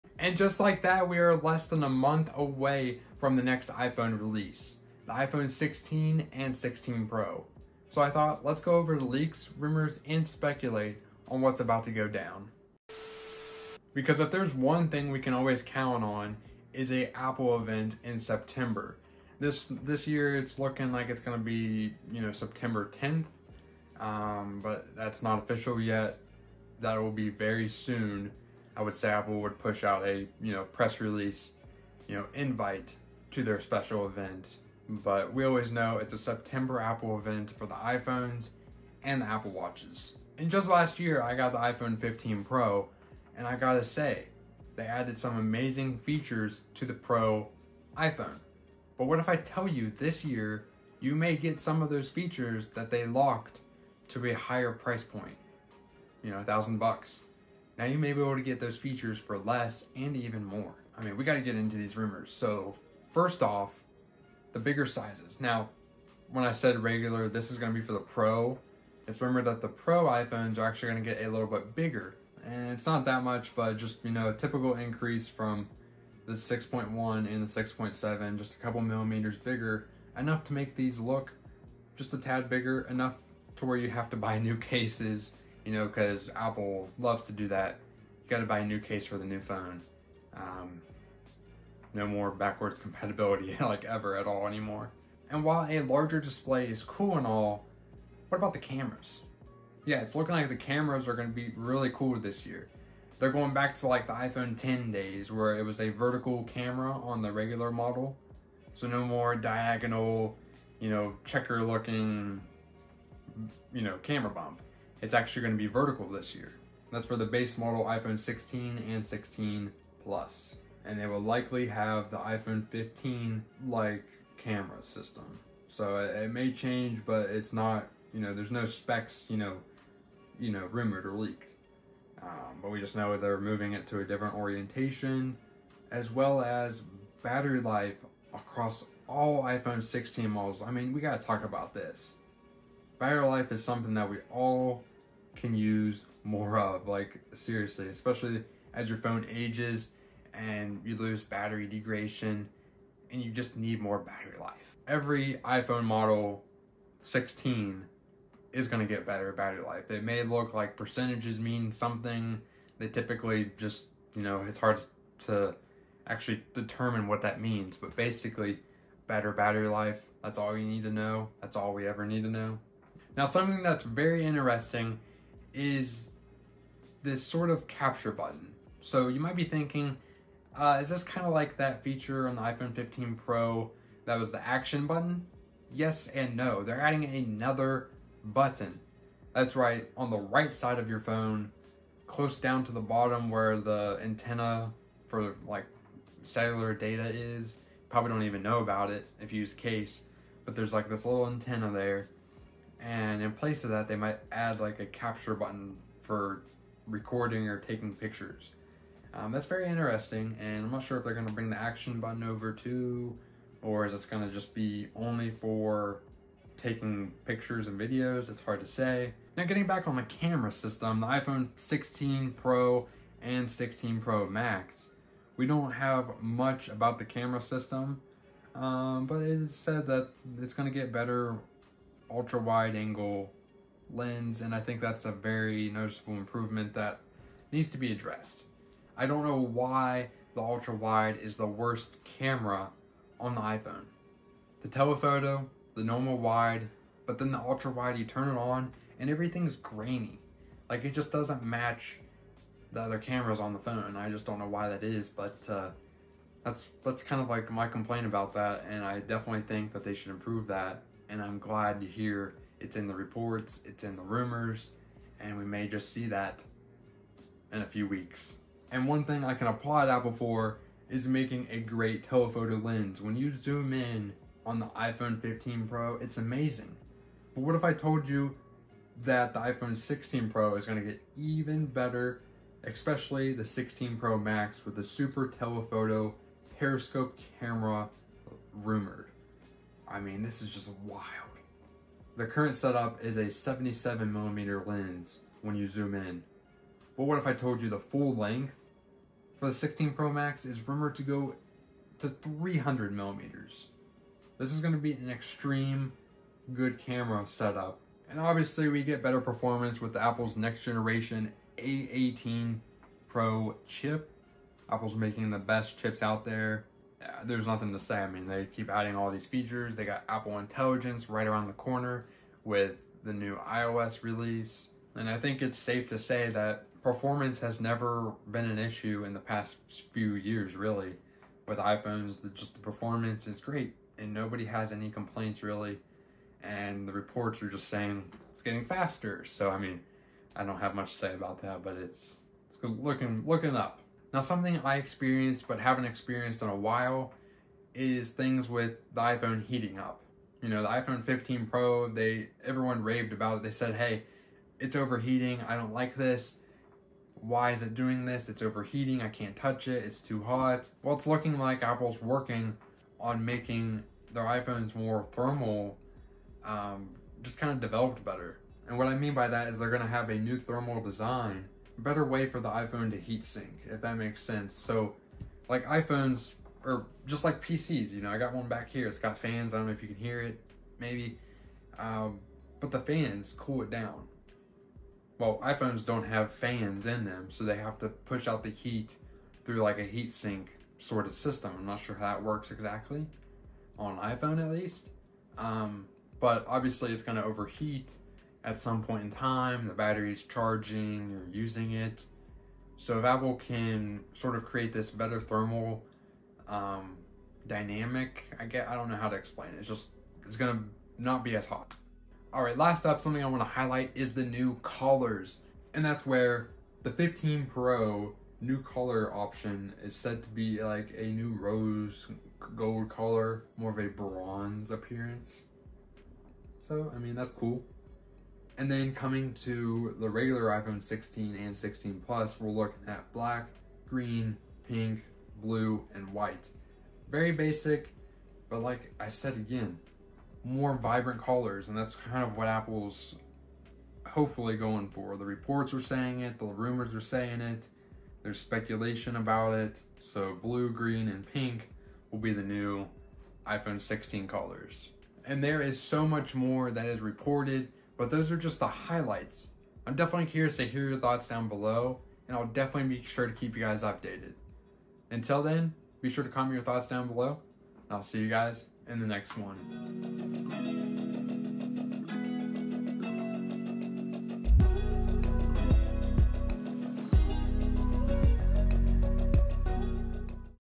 iPhone 16 Releasing Next Month! (Everything You Need to Know)
Today's video is all about leaks, rumors, and speculation on the upcoming iPhone 16 and 16 Pro, which are set to release next month. Every September, Apple releases a new iPhone, so I thought I would let you all in on what is being talked about in terms of design and features.